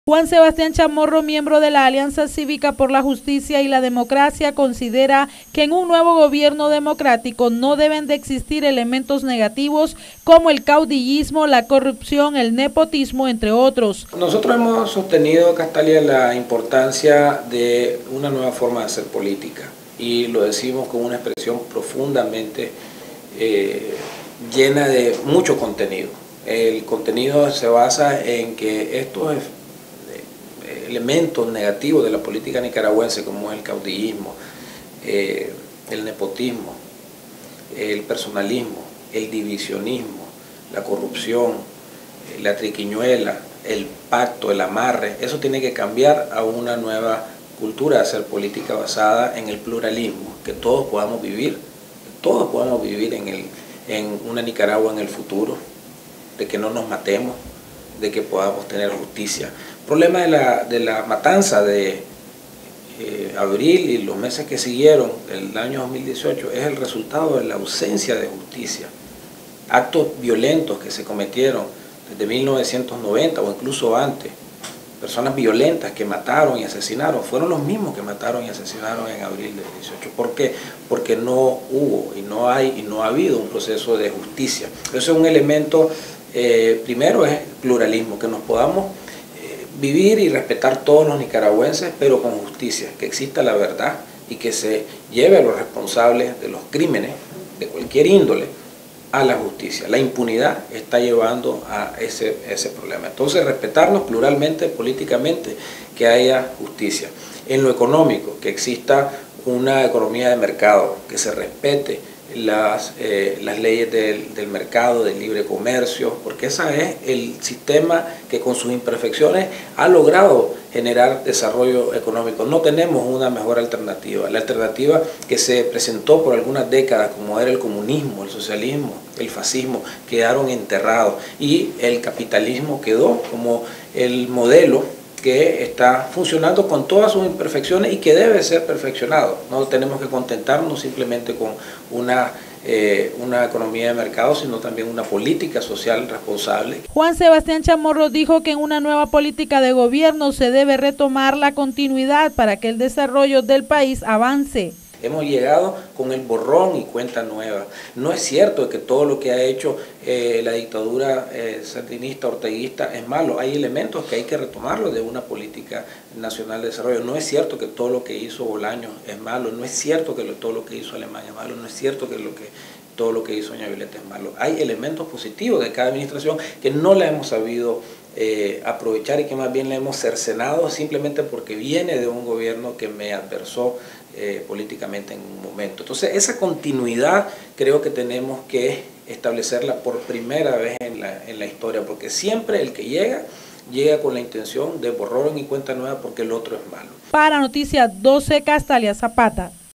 “En un nuevo gobierno no debe existir el caudillismo, los pactos ni el nepotismo”
En un nuevo gobierno no debe existir el caudillismo, los pactos ni el nepotismo, dijo Juan Sebastián Chamorro.